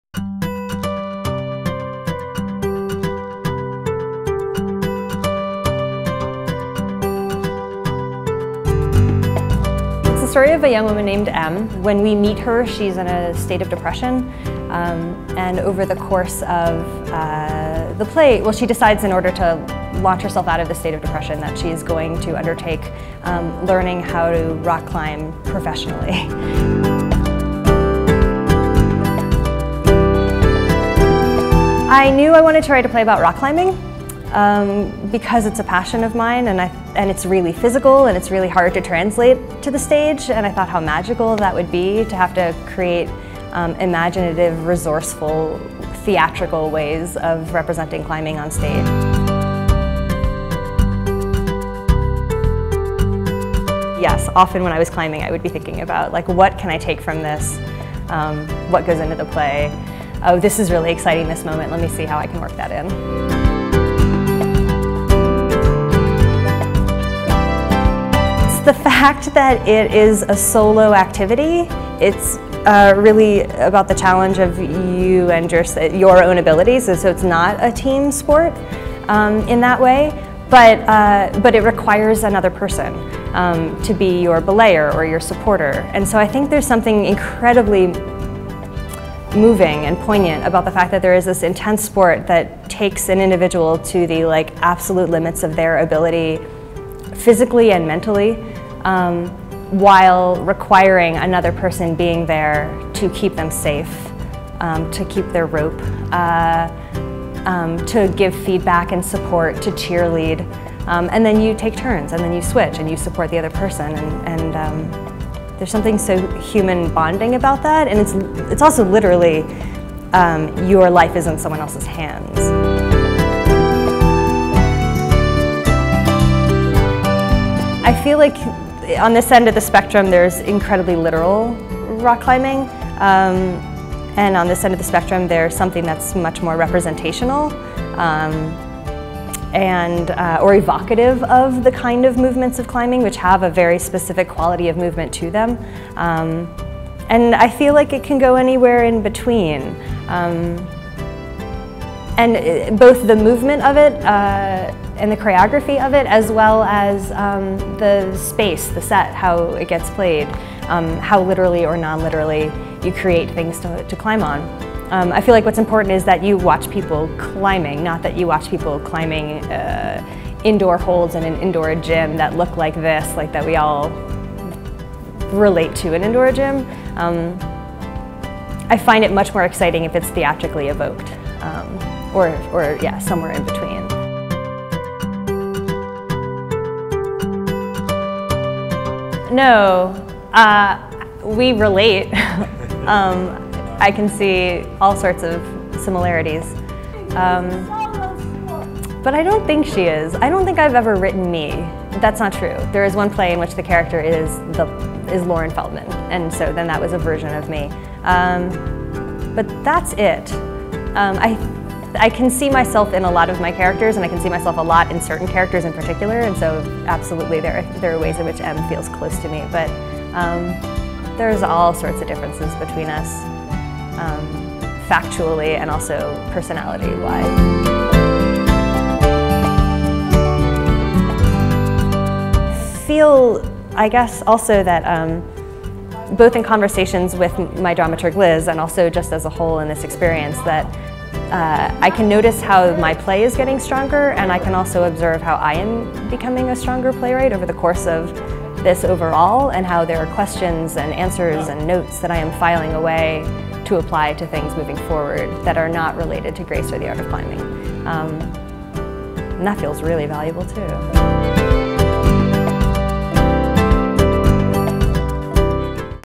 SUMMIT: Grace, Or the Art of Climbing
A new play is having a reading at the 2012 Colorado New Play Summit. It's called "Grace, Or the Art of Climbing" by Lauren Feldman. Listen to her talk about her show, and how much rock climbing has inspired her to share her thoughts in this play.